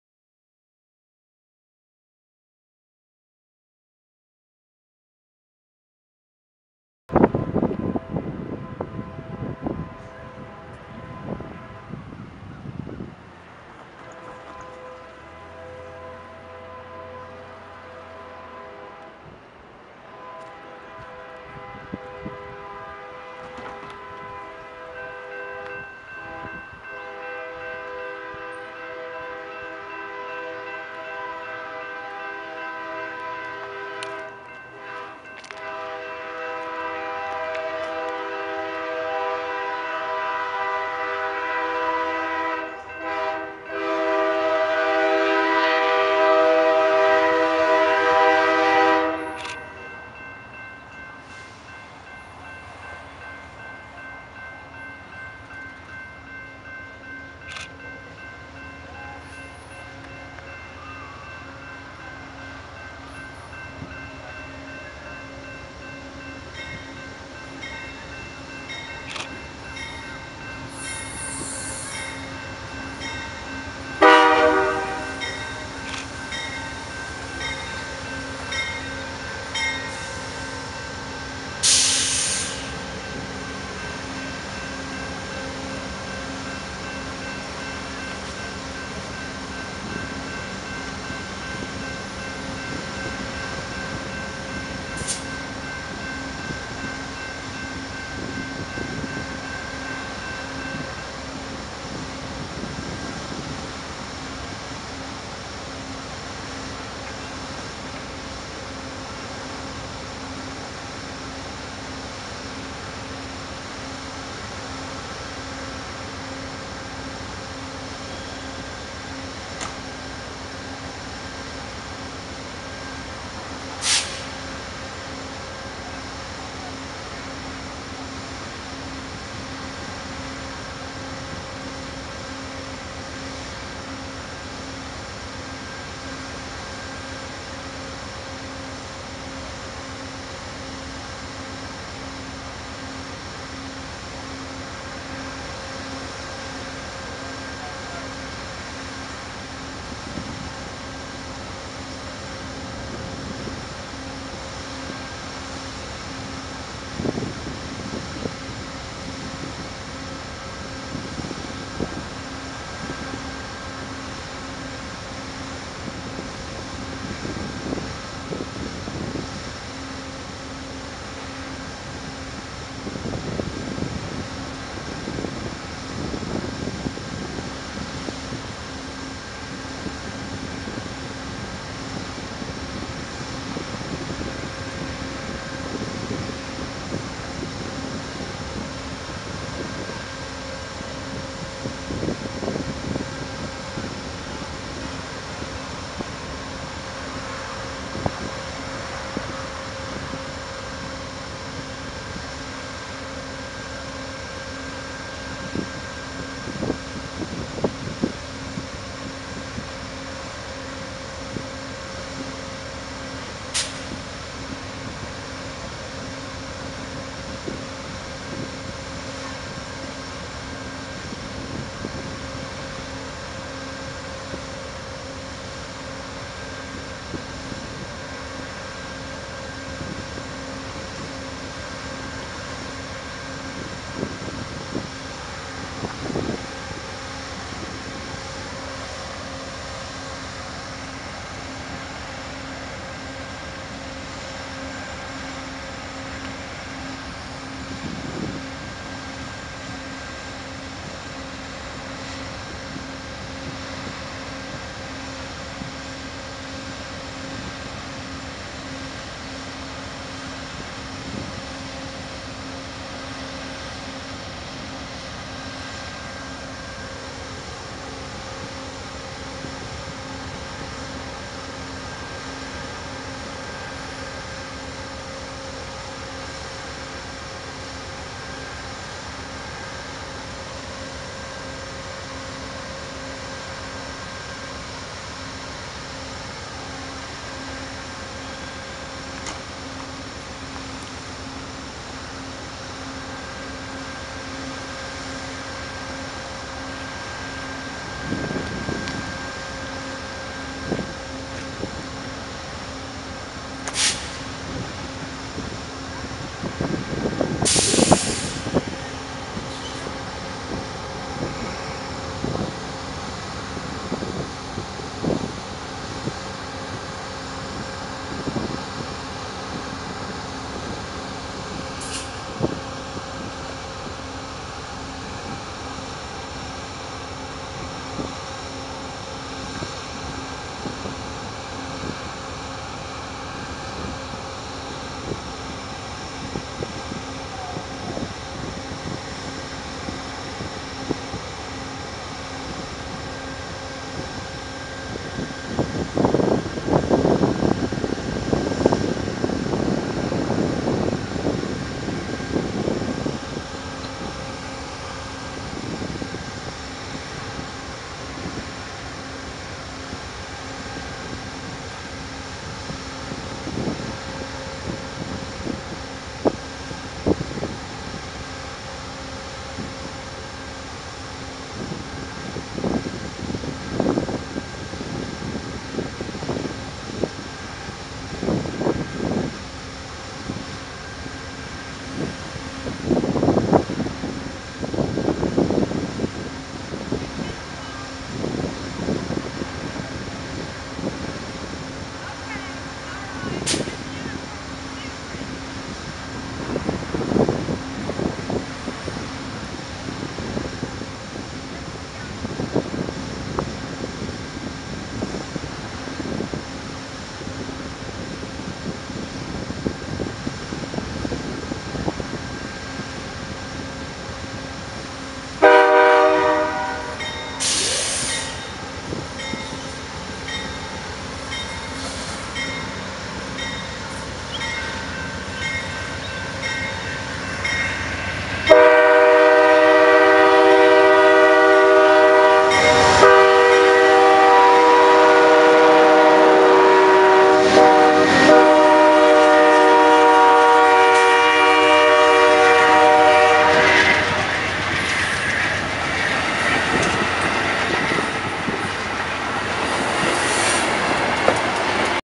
Amtrak 2016090012 03 September 2016 Heartland Flyer Norman Oklahoma By Ronald E Estes
Amtrak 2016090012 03-September-2016 Heartland Flyer 9:20am from arrival to departure All Aboard passengers boarding Texas Bound Norman Oklahoma By Ronald E Estes